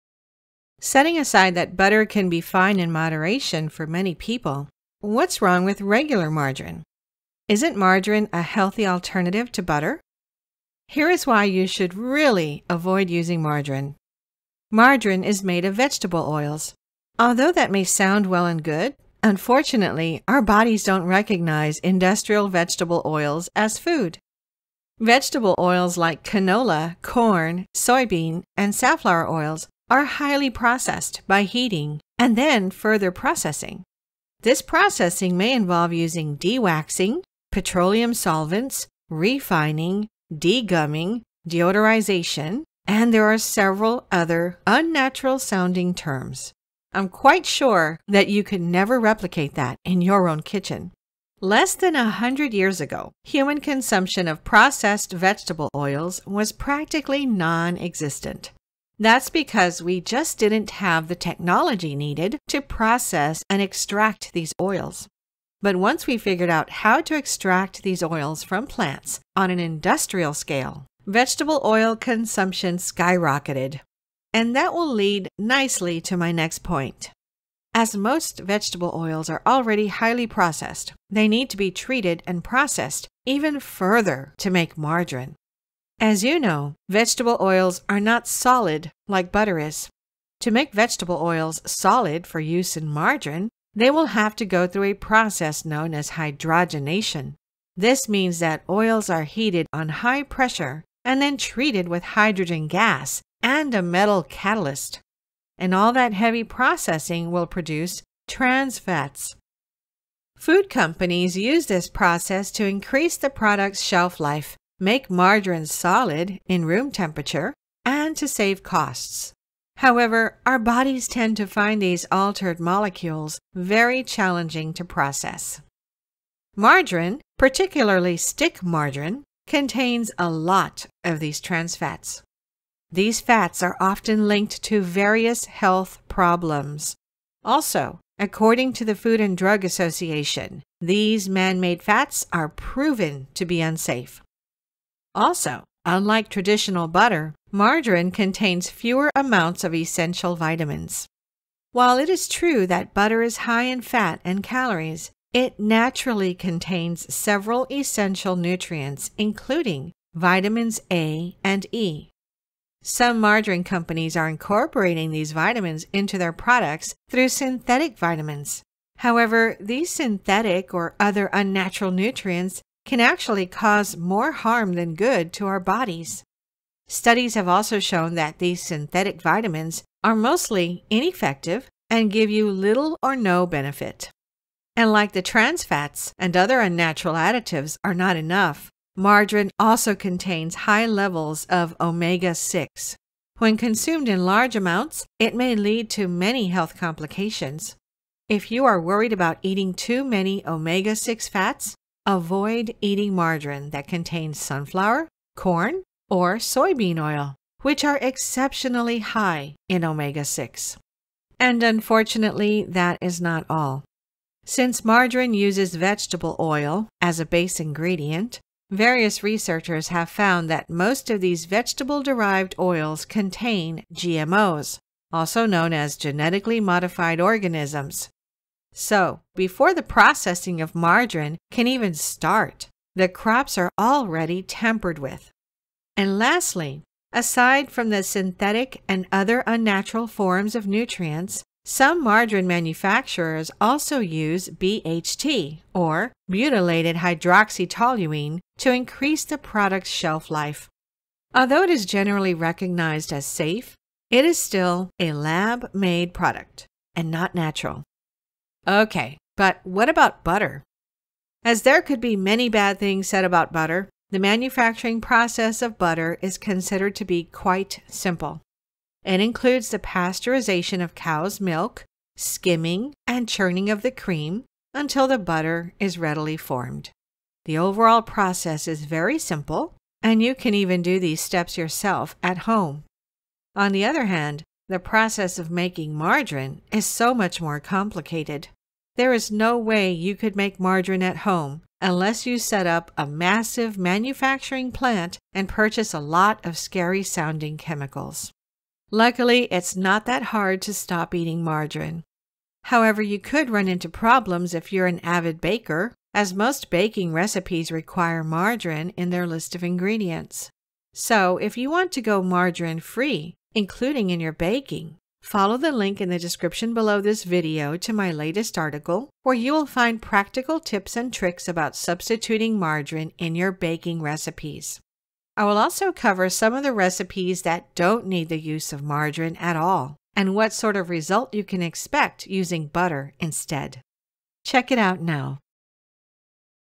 How Is Margarine Made? (And Why I Stopped Eating It)
Setting aside that butter can be fine in moderation for many people, but what's wrong with regular margarine? Isn't margarine a healthy alternative to butter?

How Is Margarine Manufactured and Here Is Why You Should Really Avoid Using Margarine.

Margarine is made of vegetable oils. Although that may sound well and good, unfortunately, our bodies don't recognize industrial vegetable oils as food.

Vegetable oils like canola, corn, soybeans, and safflower oils are highly processed by heating and then further processing. This processing may involve using de-waxing, petroleum-solvents, refining, de-gumming, deodorization, and there are several other unnatural-sounding terms. I'm quite sure that you could never replicate that in your own kitchen.

Less than a hundred years ago, human consumption of processed vegetable oils was practically non-existent. That is because we just didn't have the technology needed to process and extract these oils. But once we figured out how to extract these oils from plants on an industrial scale, the vegetable oil consumption skyrocketed.

And that will lead nicely to my next point.

As most vegetable oils are already highly processed, they need to be treated and processed even further to make margarine.

As you know, vegetable oils are not solid like butter is. To make vegetable oils solid for use in margarine, they will have to go through a process known as hydrogenation. This means that oils are heated on high pressure and then treated with hydrogen gas and a metal catalyst. And all that heavy processing will produce trans fats.

Food companies use this process to increase the product's shelf life, make margarine solid on room temperature, and save costs. However, our bodies tend to find these altered molecules very challenging to process.

Margarine, particularly stick margarine, contains a lot of these trans fats. These fats are often linked to various health problems. Also, according to the Food and Drug Association, these man-made fats are proven to be unsafe.

Also, unlike traditional butter, margarine contains fewer amounts of essential vitamins. While it is true that butter is high in fat and calories, it naturally contains several essential nutrients, including vitamins A and E.

Some margarine companies are incorporating these vitamins into their products through synthetic vitamins. However, these synthetic or other unnatural nutrients can actually cause more harm than good to our bodies. Studies have also shown that these synthetic vitamins are mostly ineffective and give you little or no benefit.

And like the trans fats, and other unnatural additives are not enough, margarine also contains high levels of omega 6. When consumed in large amounts, it may lead to many health complications. If you are worried about eating too many omega-6 fats, avoid eating margarine that contains sunflower, corn, or soybean oil, which are exceptionally high in omega 6.

And unfortunately, that is not all. Since margarine uses vegetable oil as a base ingredient, various researchers have found that most of these vegetable-derived oils contain GMOs, also known as genetically modified organisms.

So, before the processing of margarine can even start, the crops are already tampered with.

And lastly, aside from the synthetic and other unnatural forms of nutrients, some margarine manufacturers also use BHT or Butylated Hydroxytoluene to increase the product's shelf life. Although it is generally recognized as safe, it is still a lab-made product and not natural.

Ok, but what about butter?

As there could be many bad things said about butter, the manufacturing process of butter is considered to be quite simple.

It includes the pasteurization of cow's milk, skimming, and churning of the cream, until the butter is readily formed. The overall process is very simple, and you can even do these steps yourself at home.

On the other hand, the process of making margarine is so much more complicated. There is no way you could make margarine at home unless you set up a massive manufacturing-plant and purchase a lot of scary-sounding chemicals.

Luckily it's not that hard to stop eating margarine. However, you could run into problems if you are an avid baker, as most baking recipes require margarine in their list of ingredients.

So, if you want to go margarine free, including in your baking, follow the link at the beginning of this description below where you will find practical tips and tricks about substituting margarine in your baking recipes.

I will also cover some of the recipes that don't need the use of margarine at all, and what sort of result you can expect using butter instead.

Jen Evansy